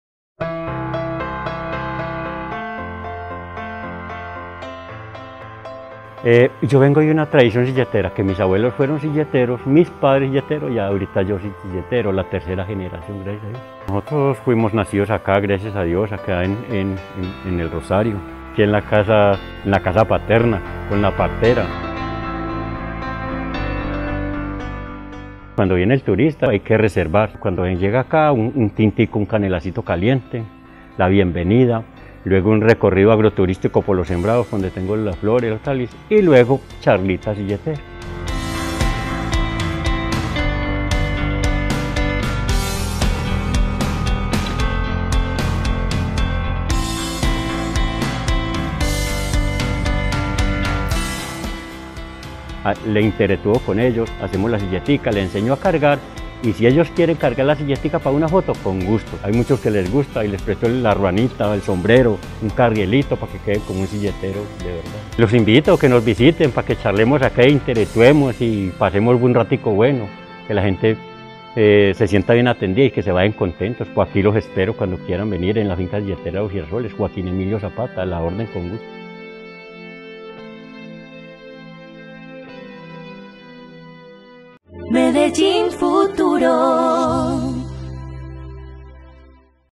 ¡La Finca silletera Los Girasoles espera por ti!
Las Fincas Silleteras son un punto turístico destacado de nuestra región. La Finca 🌻’Los Girasoles’🌻 ha mantenido la cultura silletera por tres generaciones y durante todo el año abre sus puertas para compartirla con el mundo.

🌸¡Nuestros silleteros esperan por ti!🌸